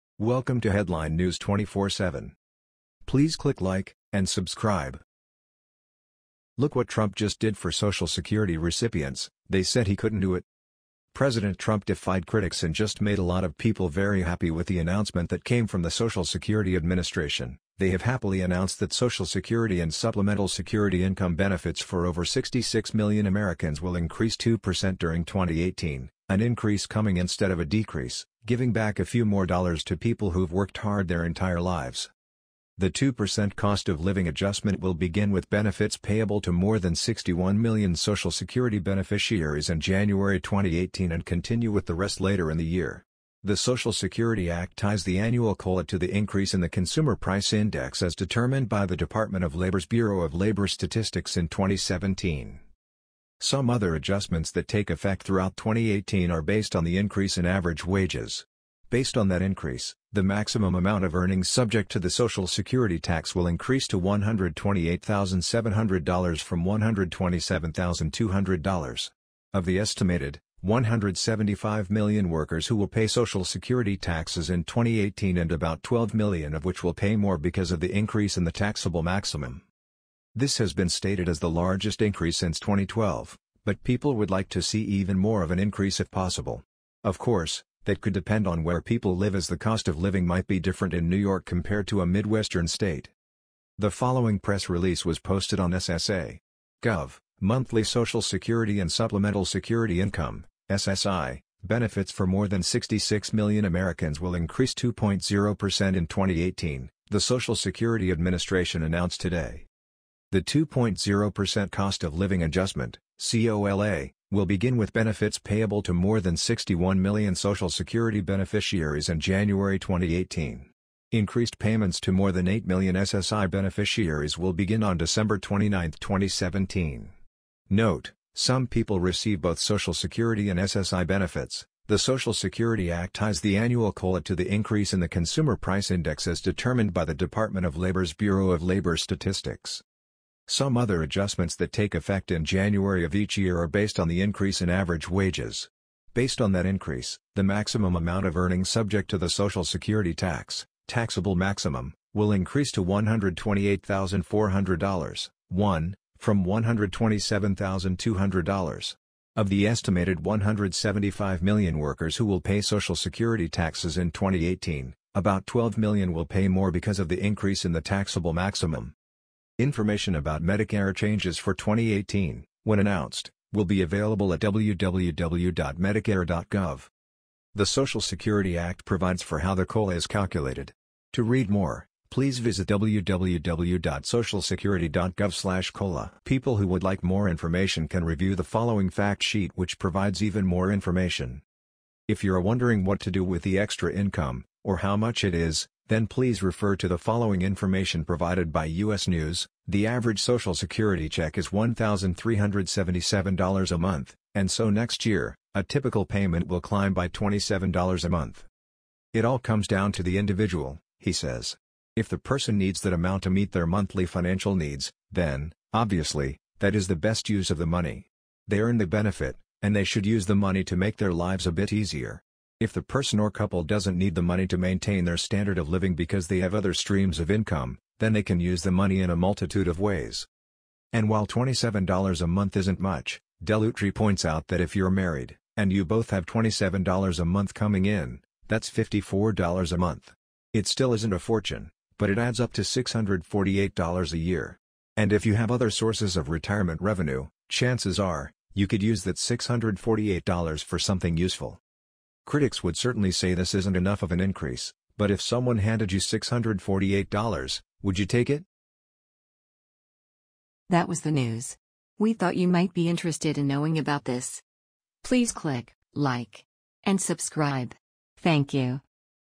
Look What Trump Just Did For Social Security Recipients — They Said He Couldn’t Do It!
Look What Trump Just Did For Social Security Recipients — They Said He Couldn’t Do It!(keywords:2% increase)
They have happily announced that Social Security and Supplemental Security Income benefits for over 66 million Americans will increase 2 percent during 2018, an increase coming instead of a decrease, giving back a few more dollars to people who’ve worked hard their entire lives.